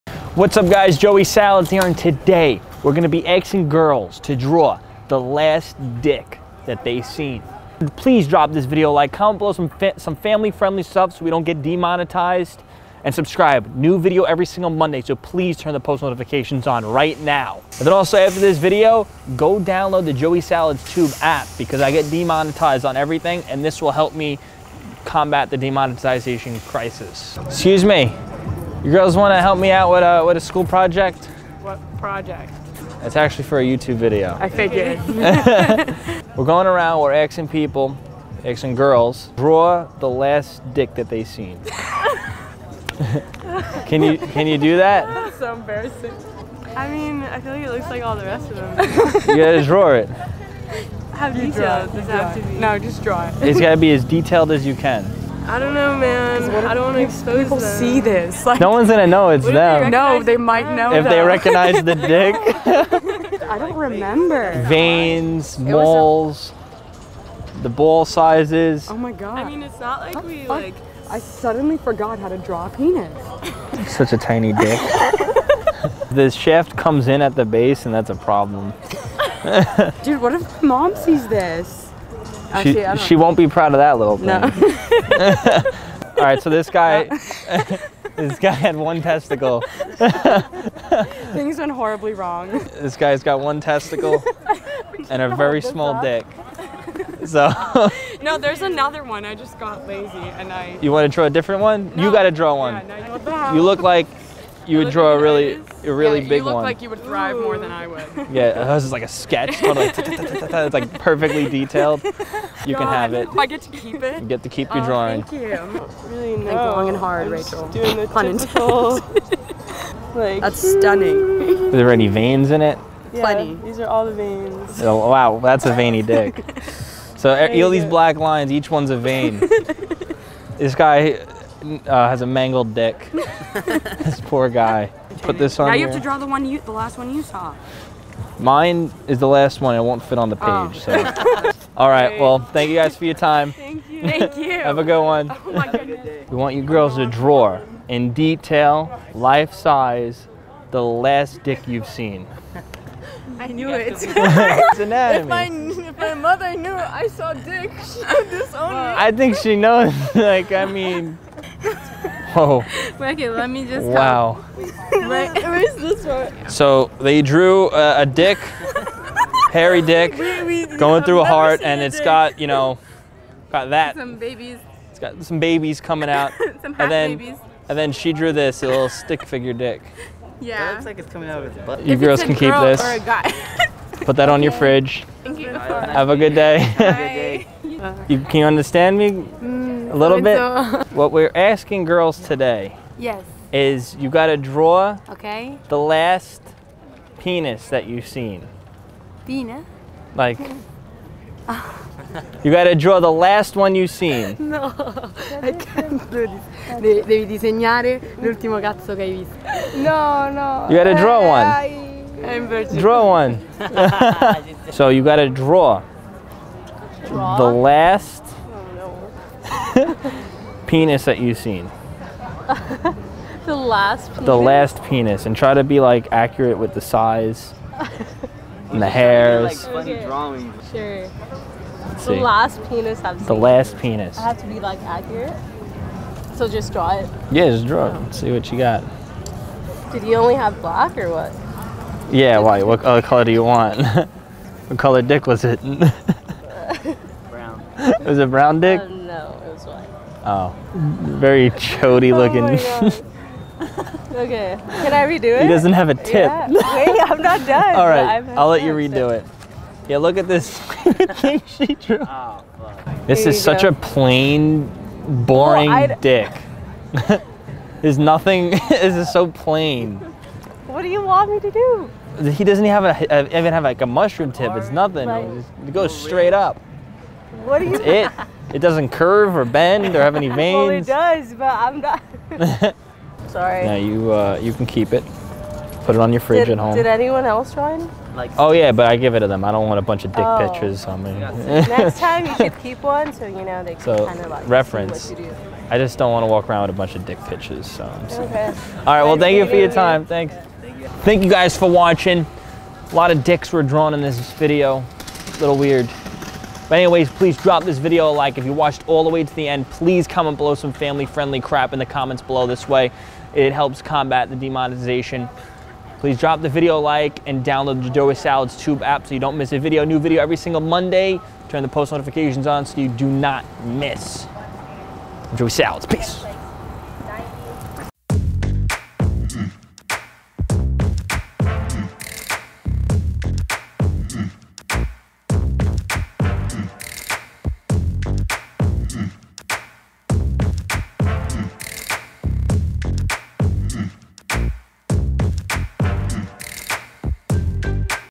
COLLEGE GIRLS DRAW DICKS (Social Experiment)
Join Joey Salads as he hits the streets with his latest prank video that will leave you laughing or thinking! Dive into a series of daring and hilarious social experiments where unsuspecting bystanders face unexpected situations. From heartwarming reactions to bewildering responses, witness the unpredictable human nature in action.